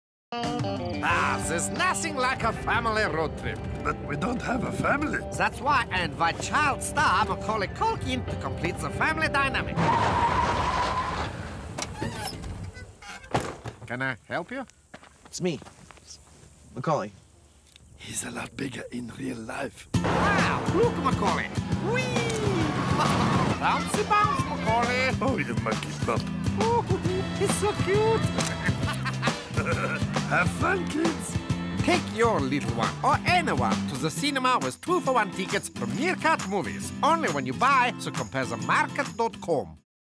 Compare the Market - Family Day Out (2016, UK)
Macauly Culkin gets invaded by another annoying duo from British advertising who just won't take him seriously as a grown man. Directed by Dave Scanlon at Passion Pictures.